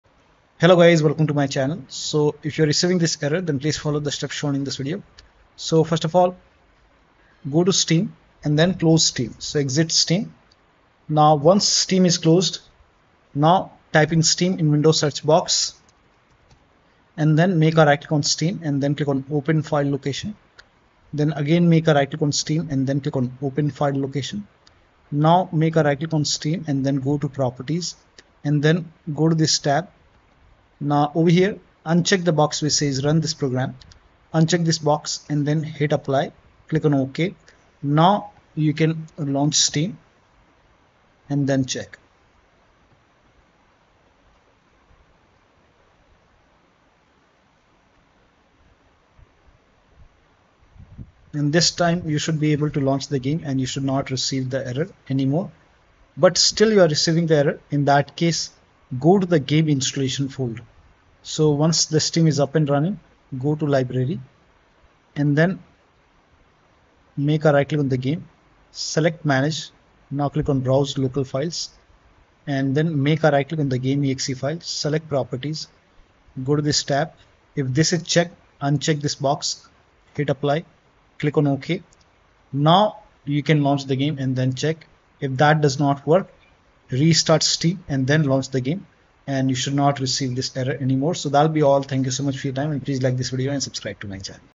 How To Fix God Of War Ragnarok Error You Need At Least Windows 10 Build 19041
Fix God Of War Ragnarok Error Version Not Supported You Need At Least Windows 10 Build 19041